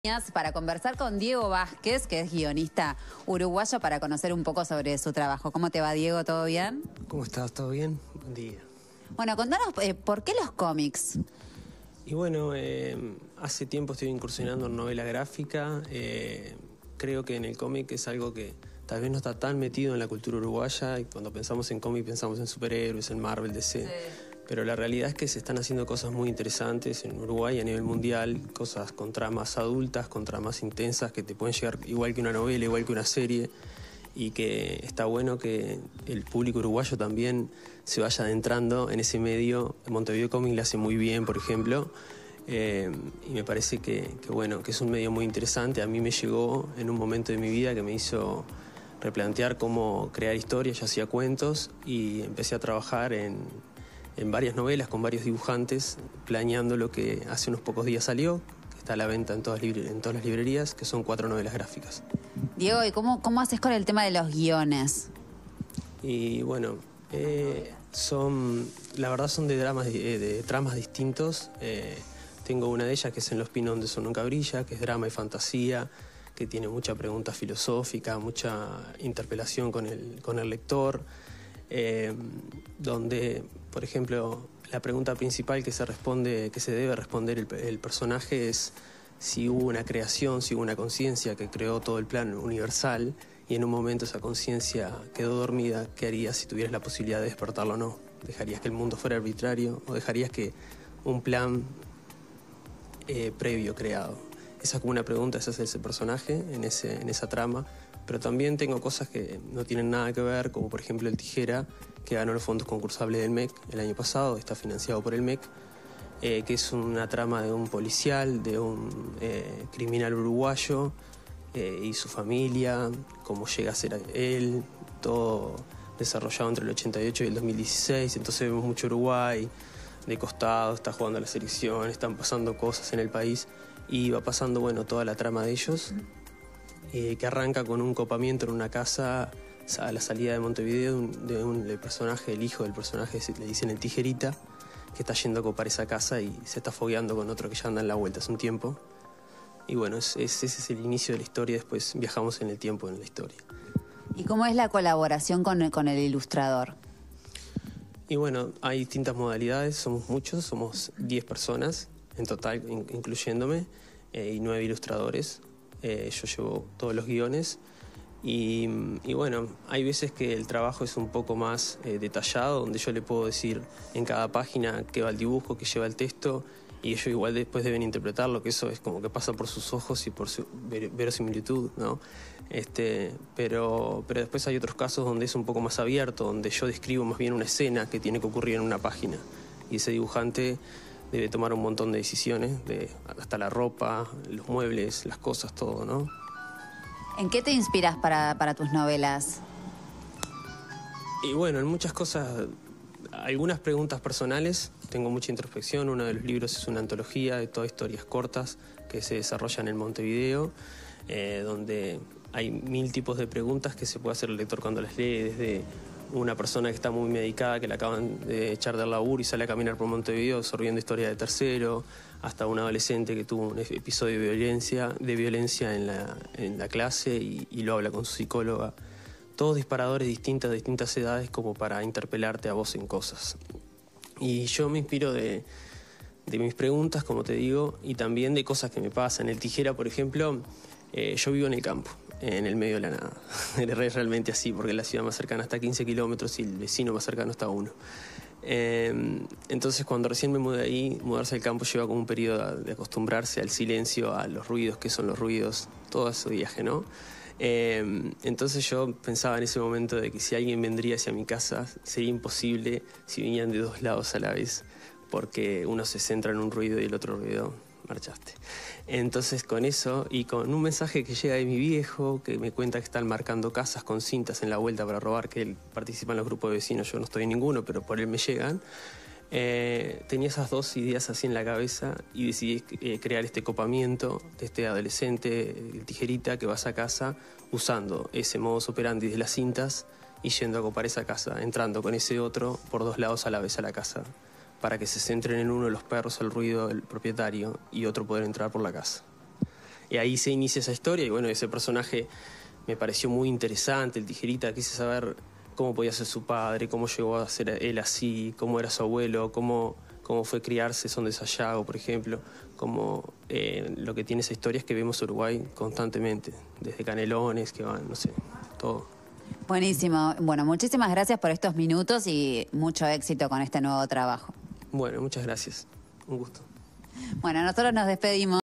Diego Vázquez: una vida dedicada a los cómics
Nadia Fumeiro conversó con el guionista uruguayo para conocer su trabajo en la novela gráfica.